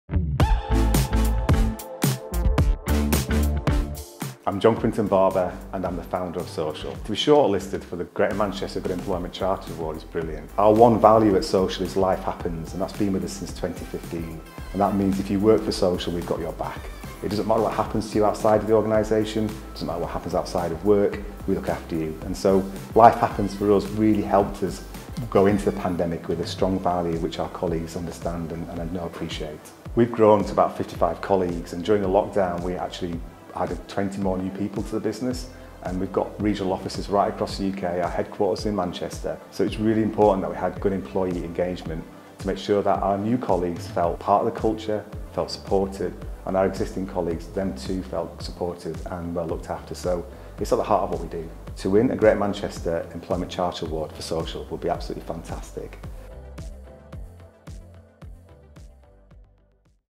Social - Best for Employee Engagement at The Greater Manchester Good Employment Awards 2022
Shortlisted for 'Best for Employee Engagement' at the Greater Manchester Good Employment Awards 2022 are... Social! Hear more about their organisation, and what winning this award would mean to them.